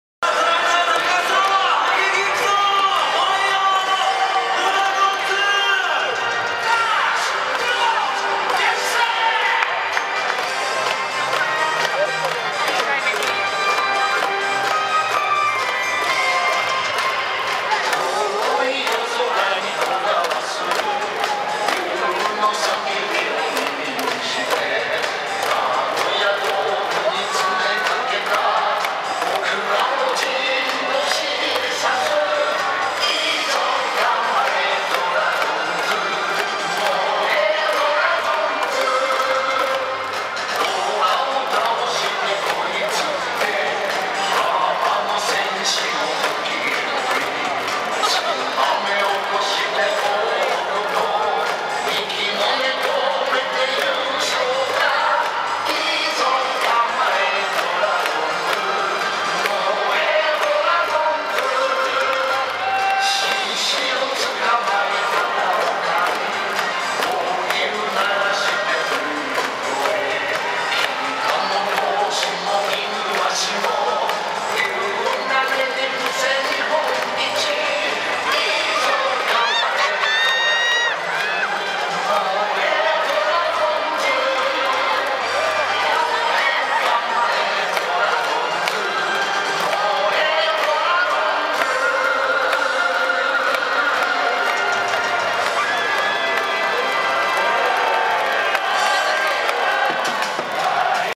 2013年9月16日 中日・藤井淳志選手応援団体観戦
中日・藤井選手を応援するために、台風の悪条件にもかかわらず日本中から大勢の藤井淳志ファンがナゴヤドームに集結しました。　藤井選手は残念ながらケガで２軍調整中ですが、間もなく１軍復帰が見込まれます。